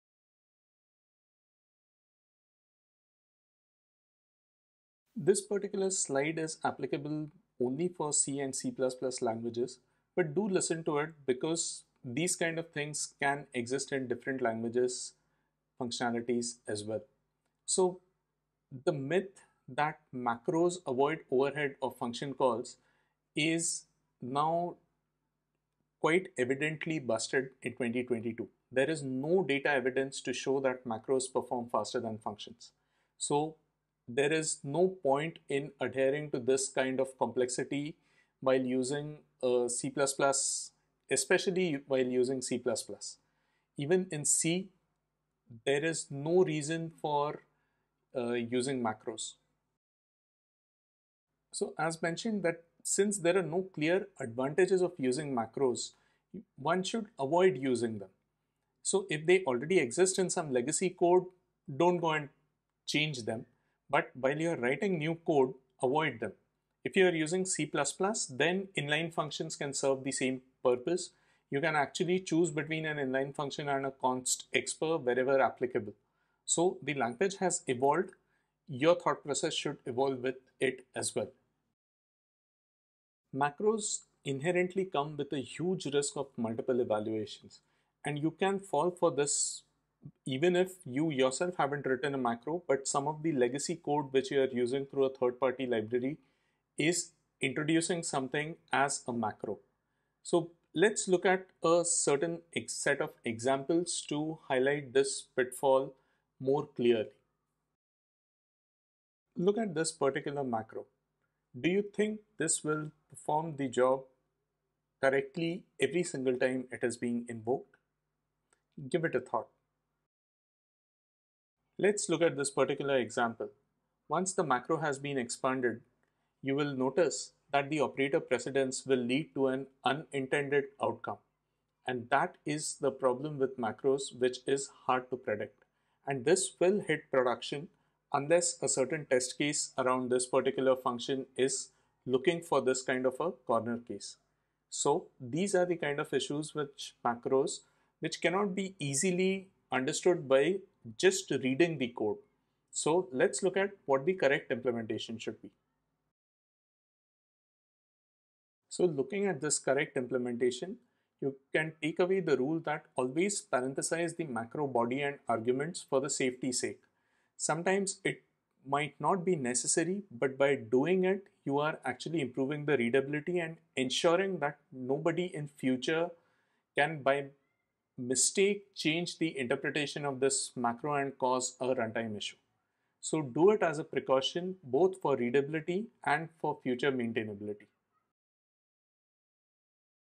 Beyond Macros: Evolving Practices in C and C++
🔍 Diving deep into C and C++ language intricacies.
🚫 Debunking myths about macros and function calls.
📅 Reflecting on the evolution of programming languages in 2022.
⚠️ Highlighting risks associated with macros.
🧐 Analyzing code examples to understand pitfalls.
🔄 Exploring the importance of operator precedence.
🛠 Offering solutions for safer and more readable code.

00:04 - Introduction to the focus on C and C++ languages.
00:15 - Debunking the myth about macros and function calls.
00:29 - The evolution of programming languages and their status in 2022.
00:45 - Risks and pitfalls associated with using macros.
01:49 - Analyzing a problematic macro example.
02:38 - Demonstrating the correct implementation for safety.
03:04 - Emphasizing the importance of parenthesizing for readability and maintainability.